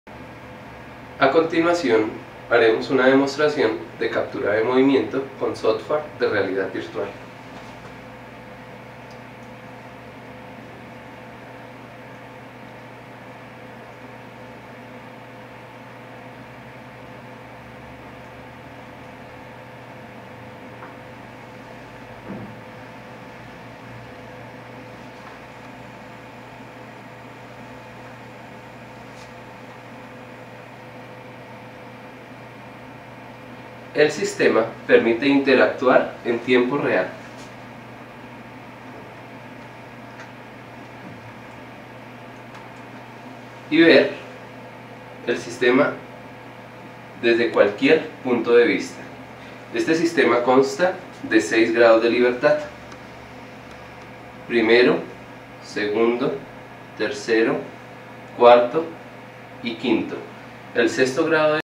brazito arm de captura de movimiento
Creado por mauricio emilio gomez y john nicholls Sistema de captura de movimiento para realidad virtual